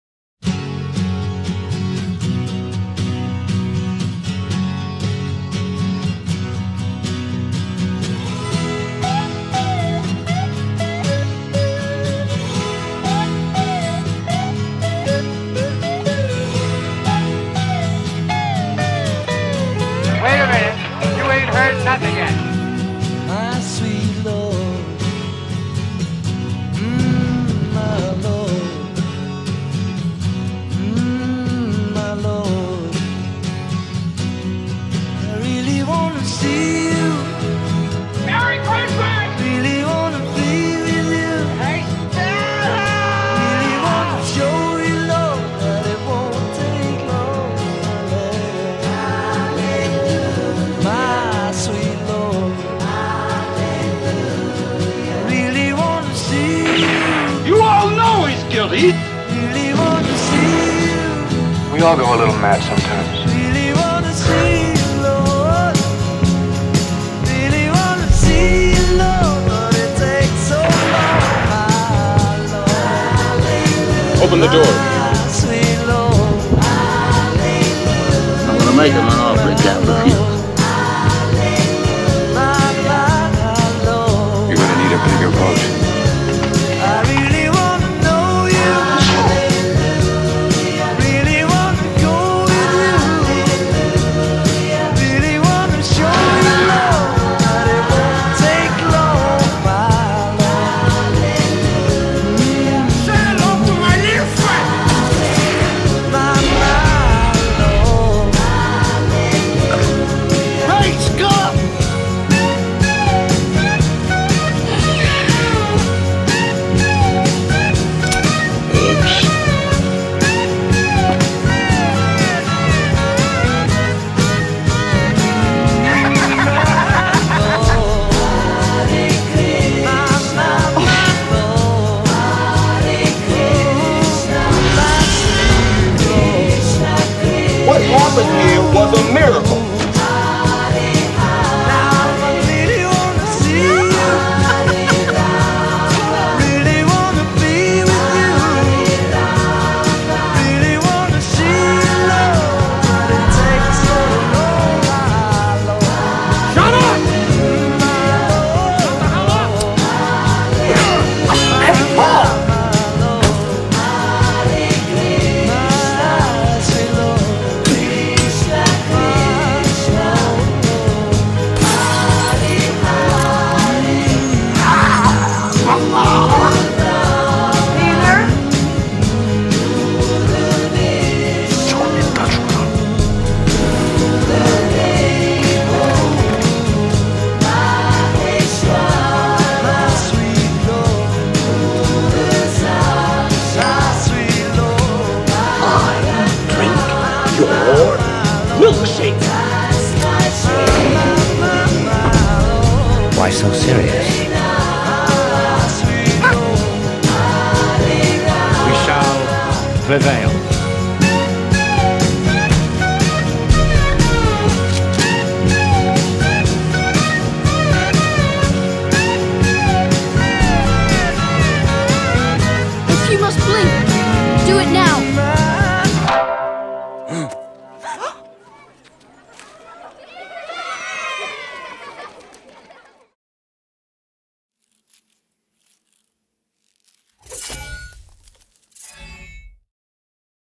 Evolution of Movies
Montage that chronicles the evolution of feature films.